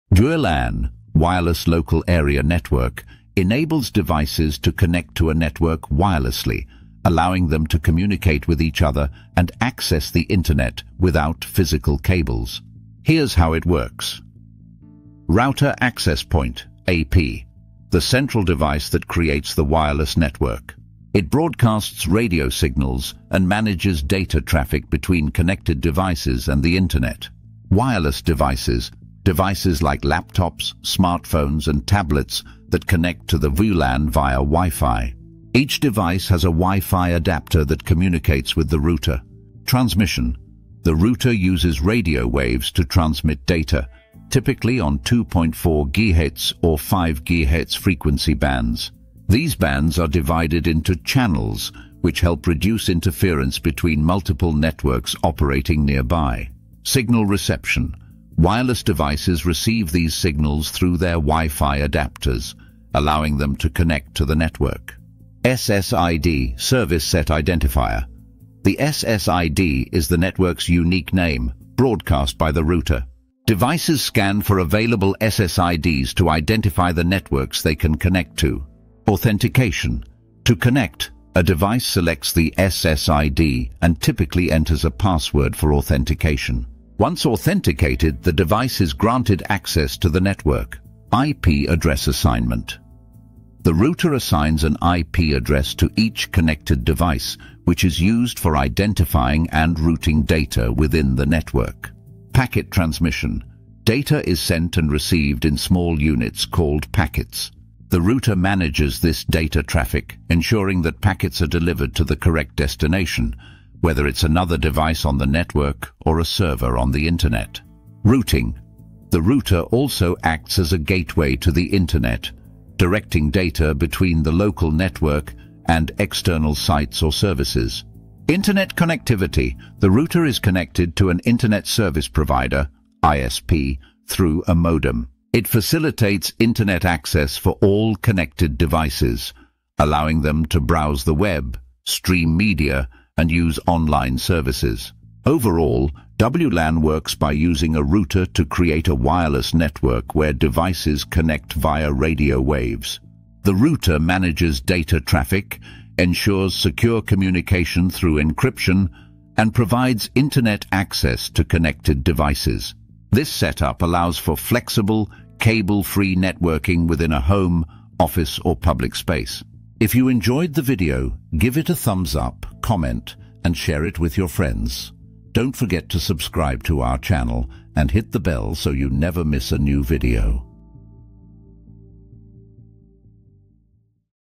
How Does WLAN Work
WLAN (Wireless Local Area Network) enables devices to connect to a network wirelessly, allowing them to communicate with each other and access the internet without physical cables. Here's how it works:

Router/Access Point (AP): The central device that creates the wireless network. It broadcasts radio signals and manages data traffic between connected devices and the internet.

Wireless Devices: Devices like laptops, smartphones, and tablets that connect to the WLAN via Wi-Fi. Each device has a Wi-Fi adapter that communicates with the router.

Transmission: The router uses radio waves to transmit data, typically on 2.4 GHz or 5 GHz frequency bands. These bands are divided into channels, which help reduce interference between multiple networks operating nearby.

Signal Reception: Wireless devices receive these signals through their Wi-Fi adapters, allowing them to connect to the network.

SSID (Service Set Identifier): The SSID is the network's unique name, broadcast by the router. Devices scan for available SSIDs to identify the networks they can connect to.

Authentication: To connect, a device selects the SSID and typically enters a password for authentication. Once authenticated, the device is granted access to the network.

IP Address Assignment: The router assigns an IP address to each connected device, which is used for identifying and routing data within the network.

Packet Transmission: Data is sent and received in small units called packets. The router manages this data traffic, ensuring that packets are delivered to the correct destination, whether it’s another device on the network or a server on the internet.

Routing: The router also acts as a gateway to the internet, directing data between the local network and external sites or services.

Internet Connectivity: The router is connected to an internet service provider (ISP) through a modem. It facilitates internet access for all connected devices, allowing them to browse the web, stream media, and use online services.

Overall, WLAN works by using a router to create a wireless network where devices connect via radio waves. The router manages data traffic, ensures secure communication through encryption, and provides internet access to connected devices. This setup allows for flexible, cable-free networking within a home, office, or public space.